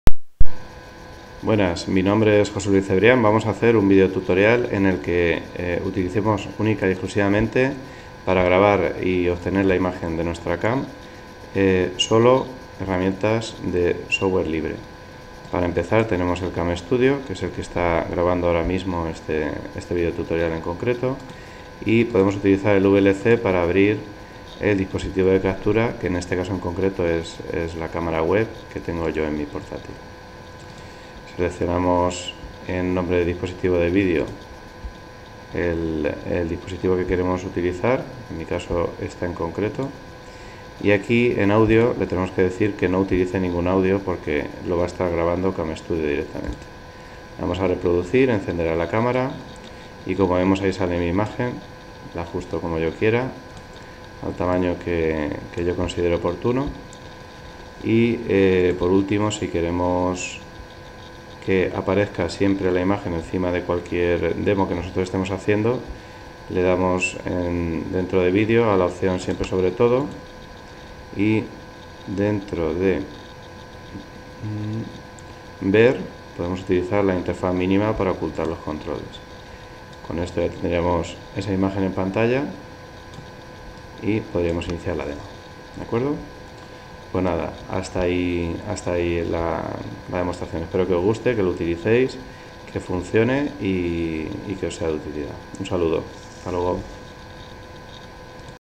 Creando demos y tutos con cam usando sofware libre
Aprende a crear tus videotutoriales y demos con tu imagen en pantalla utilizando sofware libre, sin coste alguno.

Sigue las instrucciones del vídeo.